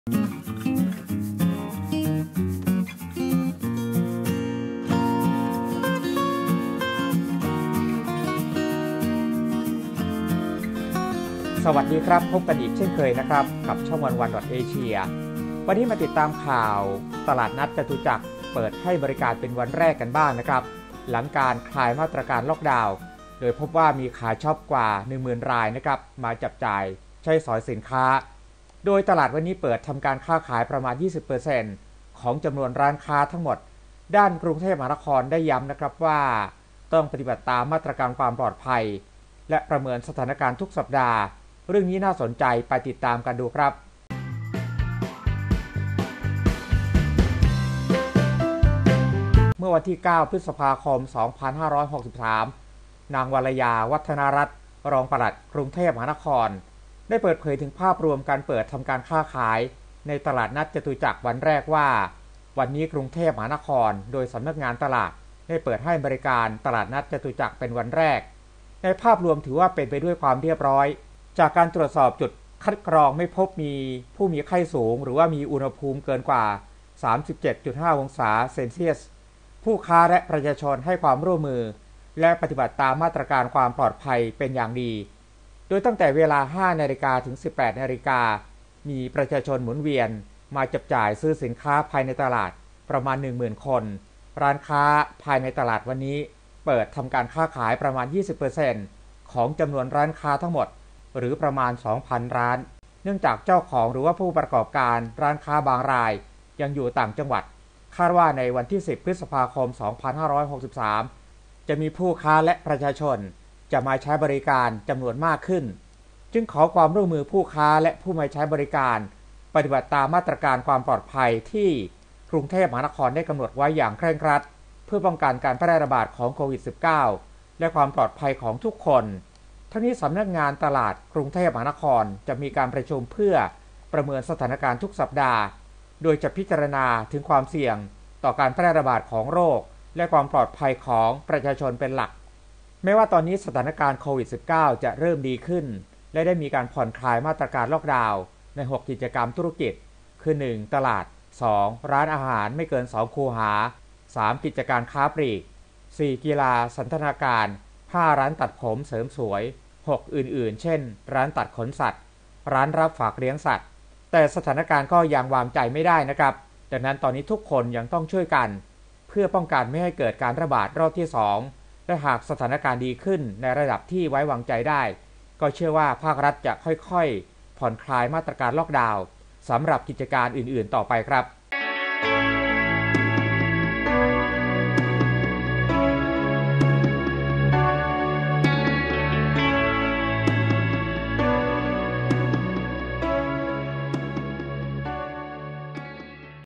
กลับมามีชีวิตชีวา เปิดแล้ว ตลาดนัดจตุจักรวันแรก
สวัสดีครับพบกันอีกเช่นเคยกับช่อง 1one.asia วันนี้มาติดตามข่าว ตลาดนัดจตุจักรเปิดให้บริการวันแรก หลังการคลายมาตรการล็อกดาวน์ โดยพบว่า ขาช้อปกว่า 10,000 รายจับจ่ายสินค้า โดยตลาดวันนี้เปิดทำการค้าขายประมาณ 20% ของจำนวนร้านค้าทั้งหมด หรือประมาณ 2,000 ร้าน ด้านกทม. ย้ำปฏิบัติตามมาตรความปลอดภัย ประเมินสถานการณ์ทุกสัปดาห์ เรื่องนี้น่าสนใจไปติดตามกันครับ คลิกอ่านรายละเอียด